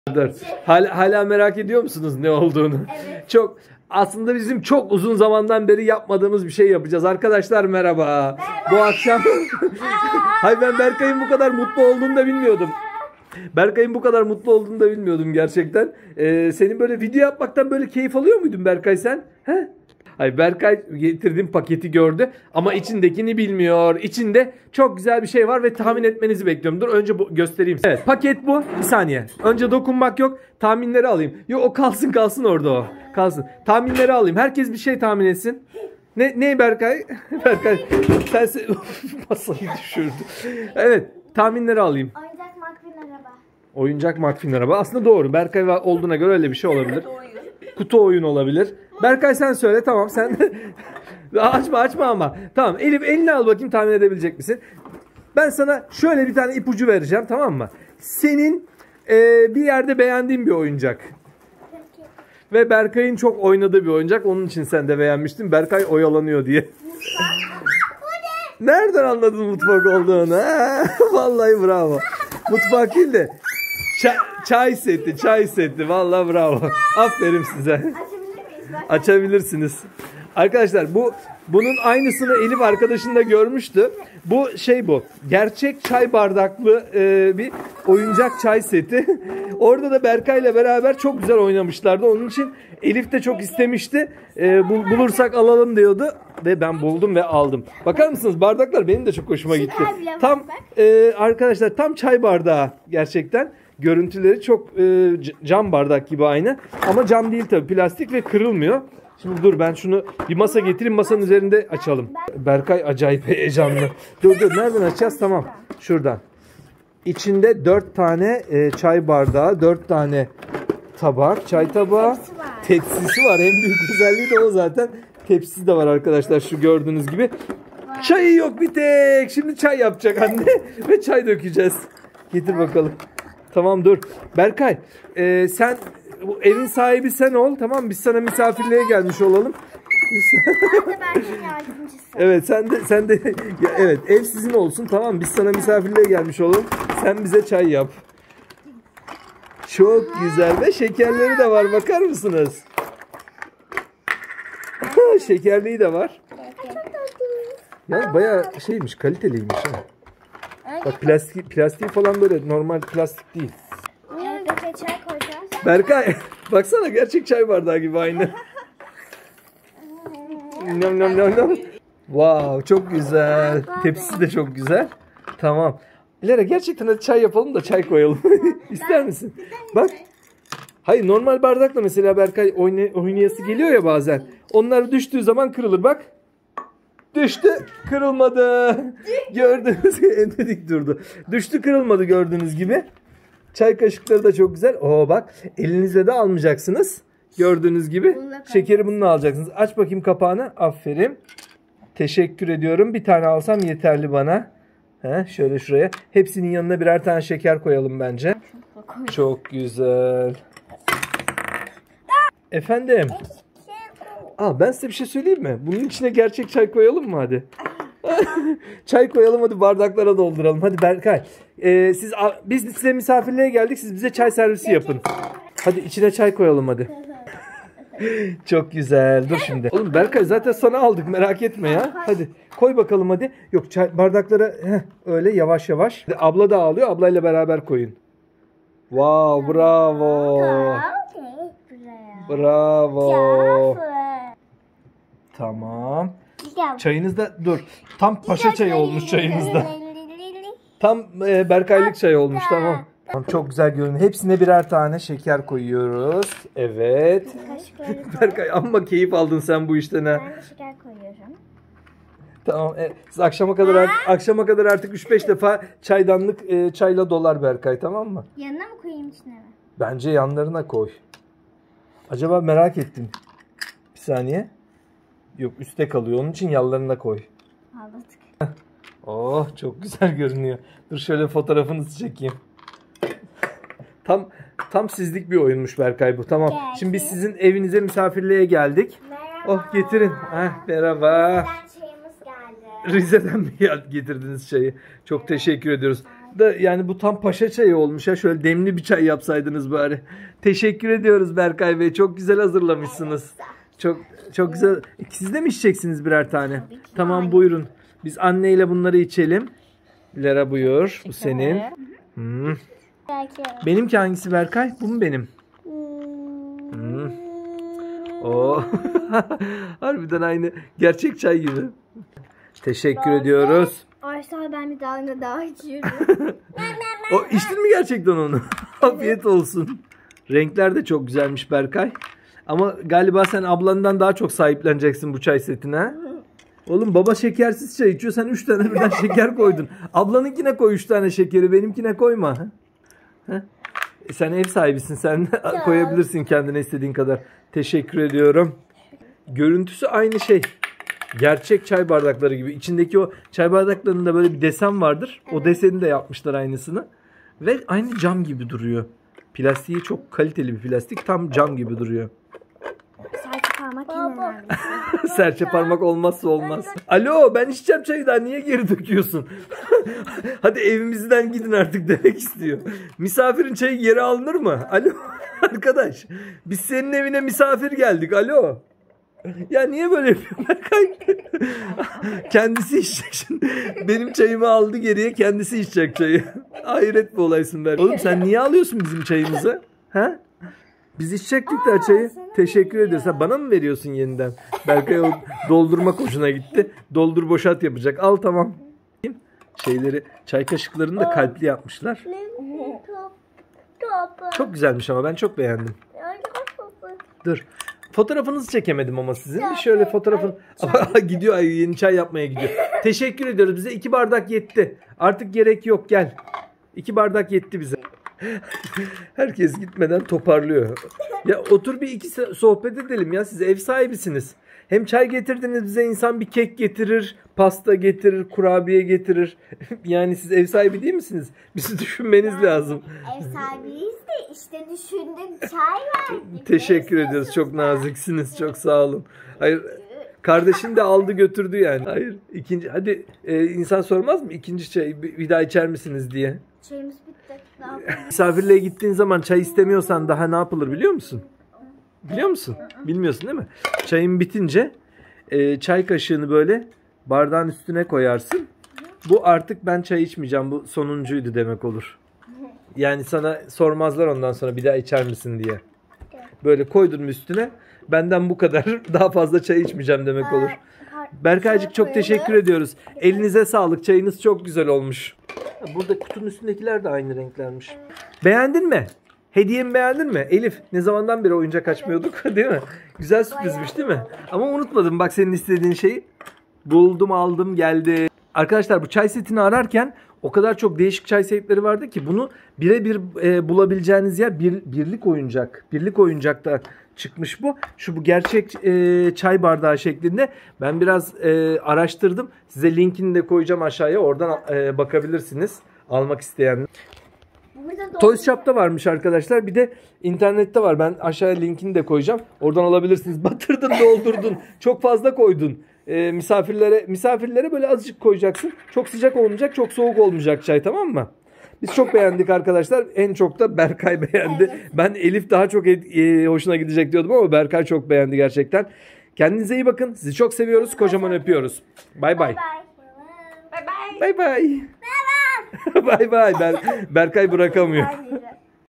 İnce Belli Gerçek Çay bardaklı Çay seti, çay vakti . Berkay Elif bu oyuncak seti çok sevdi
Arkadaşlar İnce bardaklı çay setini uygun fiyata hemen almak isterseniz linkler aşağıda;
Trendyol

Ve Toyzzshop, e-bebek, armağan oyuncak ve daha birçok oyuncak marketinde de bulabilirsiniz...
Arkadaşlar İnce belli çay bardağı setini daha detaylı incelemek isterseniz bu videoya bakabilirsiniz.